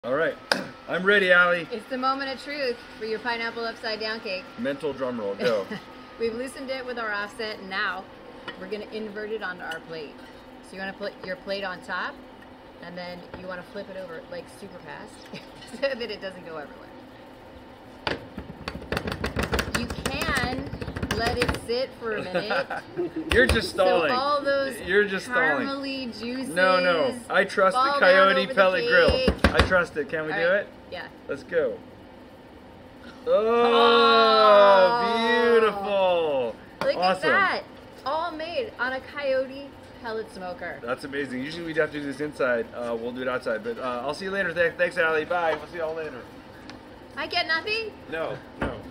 Perfect Pineapple Upside Down Cake on the Coyote Pellet Grill with Jet and Ali Tila
A perfect pineapple upside-down cake baked directly on the CoyotePelletGrill. 🍍🍰 Get Jet Tila and Ali Tila's recipe on our